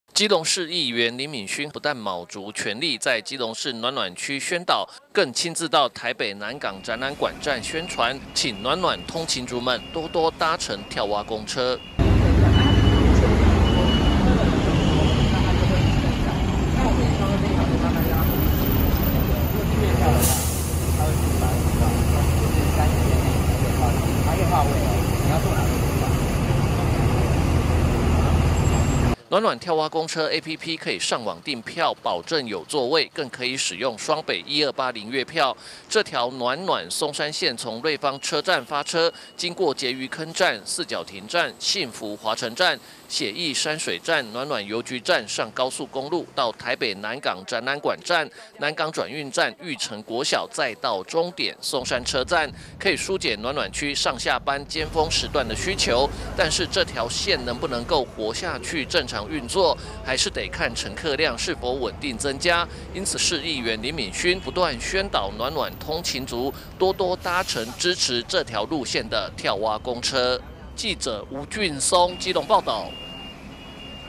新圍 基隆|暖暖跳蛙公車很方便|議員到台北南港展覽館宣傳|幫助通勤族有車子搭有位置坐車|呼籲多搭暖暖跳蛙公車 林旻勳到南港宣傳
呼籲多搭暖暖跳蛙公車 林旻勳到南港宣傳

(中嘉吉隆新聞記者吳俊松/基隆報導)為了讓暖暖”跳蛙公車”新路線，能夠活下去正常運作讓暖暖區通勤的民眾更方便，基隆市議員林旻勳不但卯足全力在基隆市暖暖區宣導，更親自到台北南港展覽館站，宣傳暖暖通勤族們多多搭乘跳蛙公車！